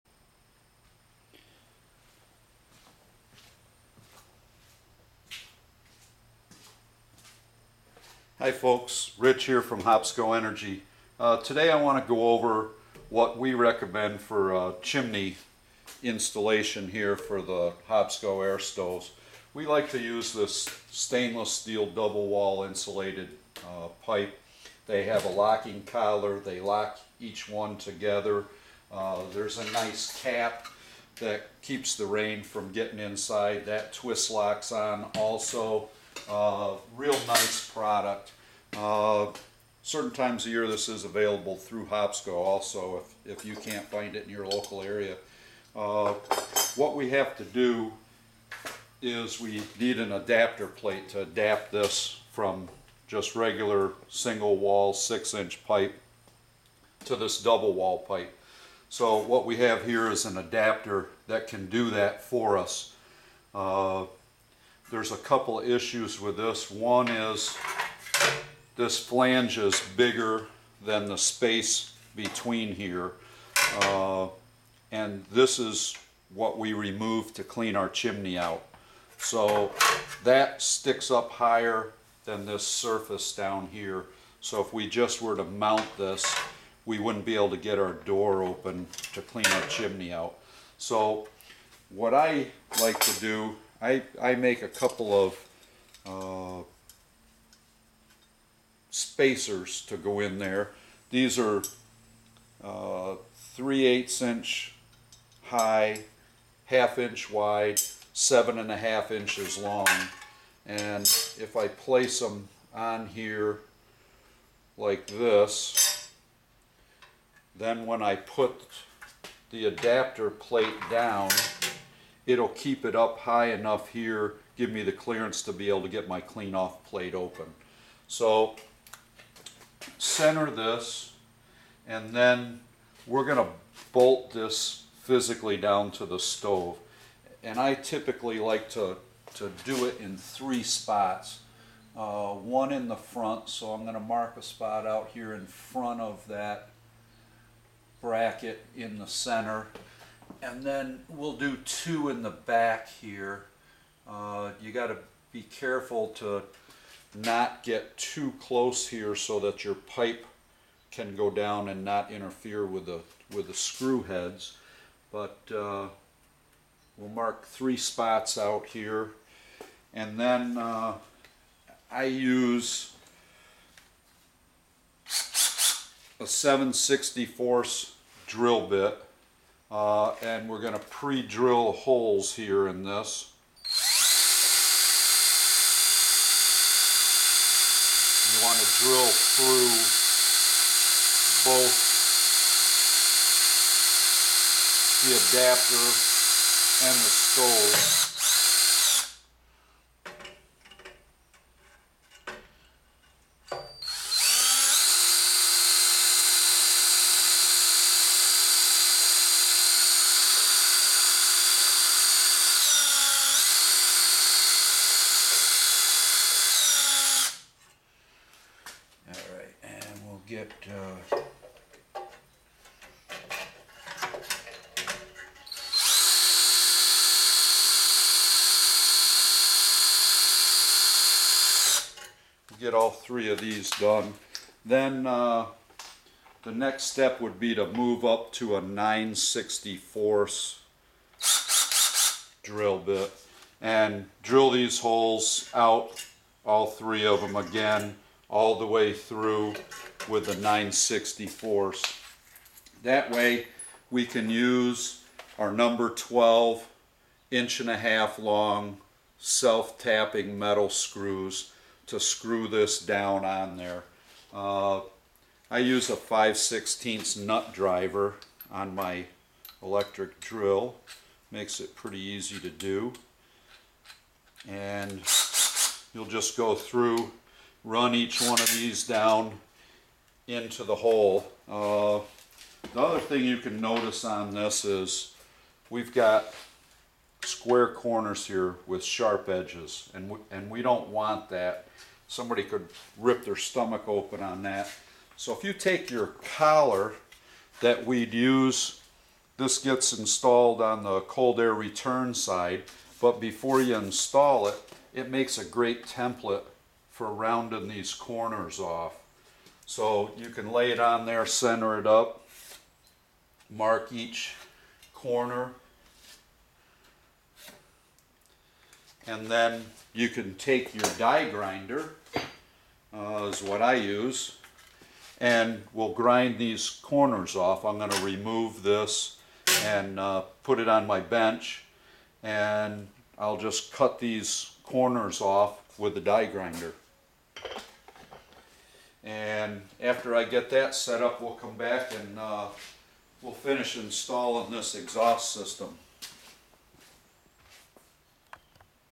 chimney vidio, Hopsco Airstove videos
Method for installing insulated SS double wall pipe on Hopsco AIRSTOVES.